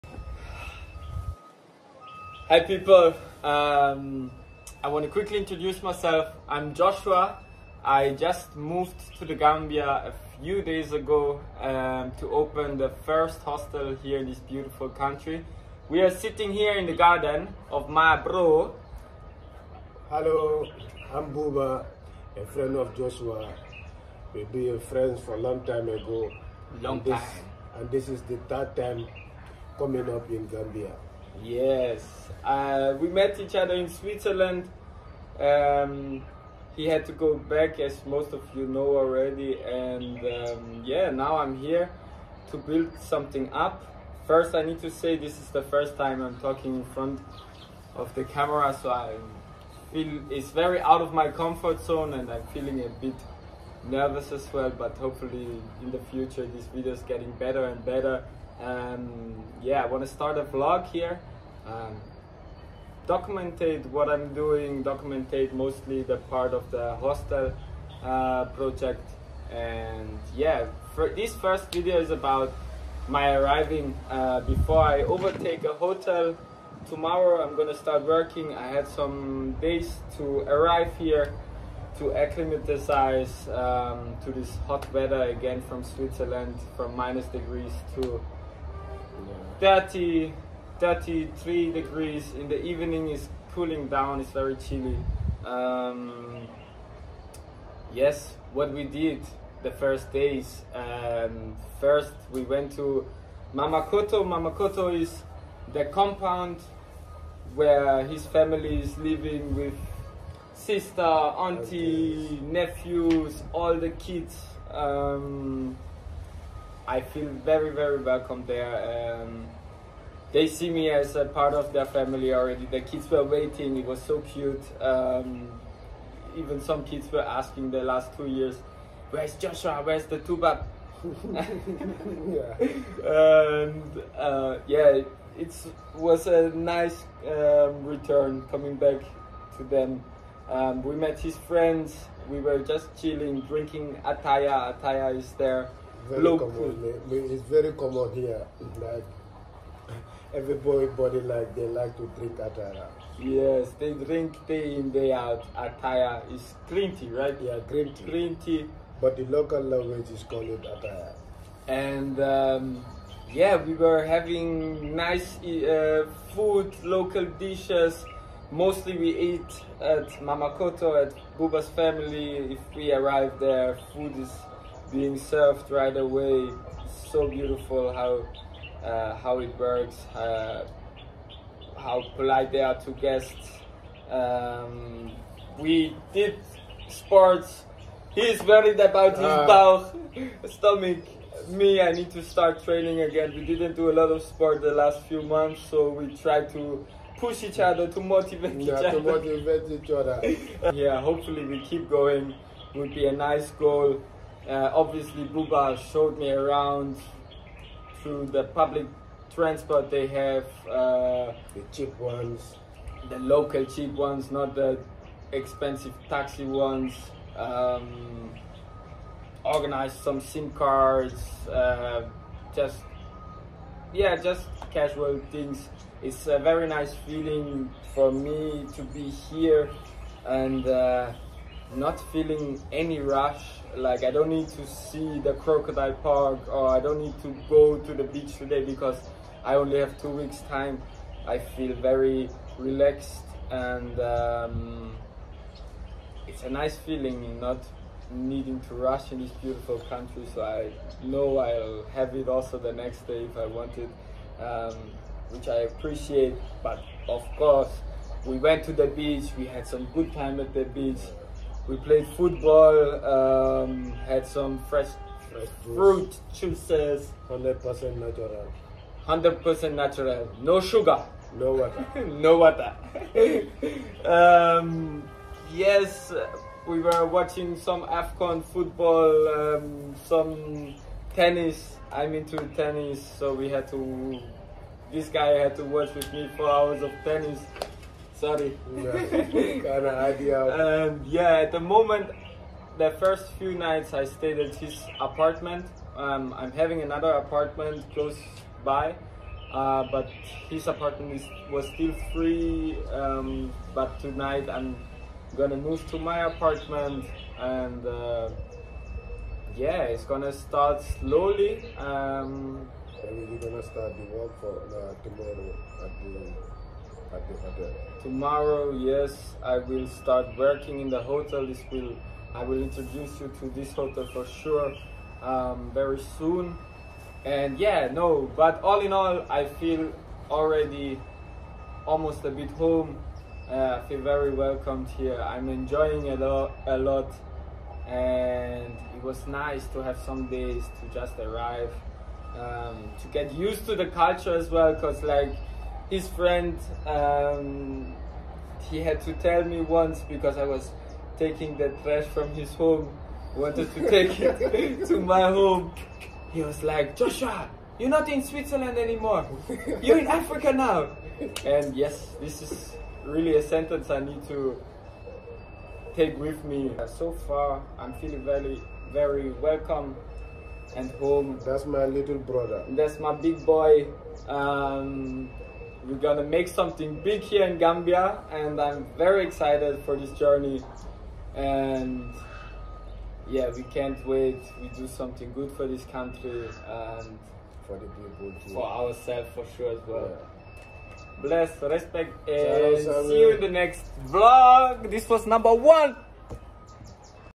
Gambia Vlog Nr 1: The arriving days
My first vlog about my arriving days after my move to The Gambia🙏🏼
Happy for some feedback so i can improve my videos:)